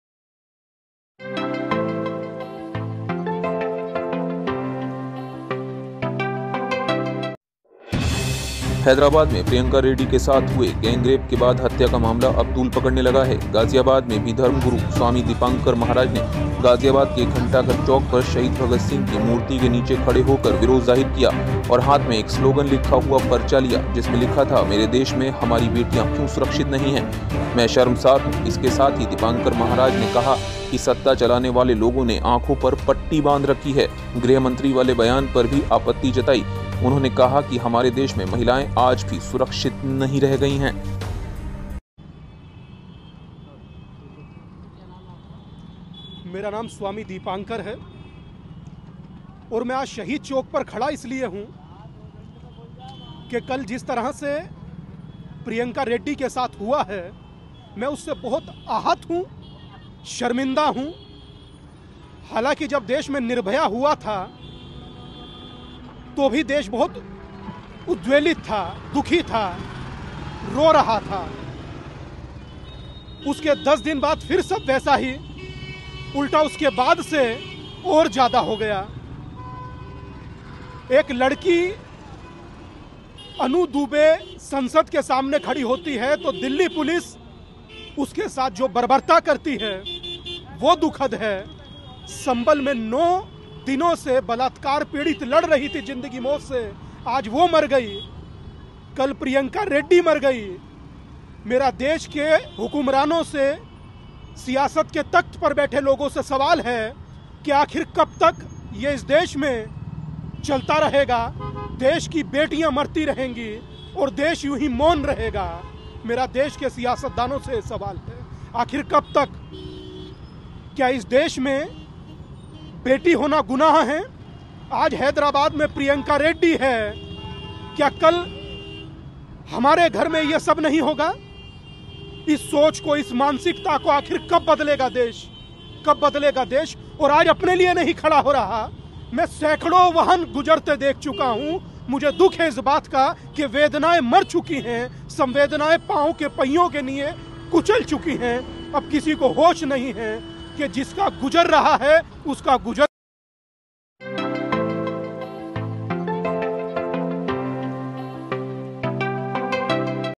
Ghaziabad: हैदराबाद की बेटी के लिए गाजियाबाद में धर्मगुरु ने किया विरोध
हैदराबाद में प्रियंका रेड्डी के साथ हुए गैंगरेप के बाद हत्या का मामला अब तूल पकड़ने लगा है गाजियाबाद में भी धर्मगुरु स्वामी दीपांकर महाराज ने गाजियाबाद के घंटाघर चौक पर शहीद भगत सिंह की मूर्ति के नीचे खड़े होकर प्रदर्शन किया और हाथ में एक स्लोगन लिखा हुआ पर्चा लिया जिसमें लिखा था मेरे देश में हमारी बेटियां क्यों सुरक्षित नहीं है मैं शर्मसार हूं। इसके साथ ही दीपांकर महाराज ने कहा कि सत्ता चलाने वाले लोगों ने आंखों पर पट्टी बांध रखी है गृहमंत्री वाले बयान पर भी आपत्ति जताई कहा कि हमारे देश में महिलाएं आज भी सुरक्षित नहीं है.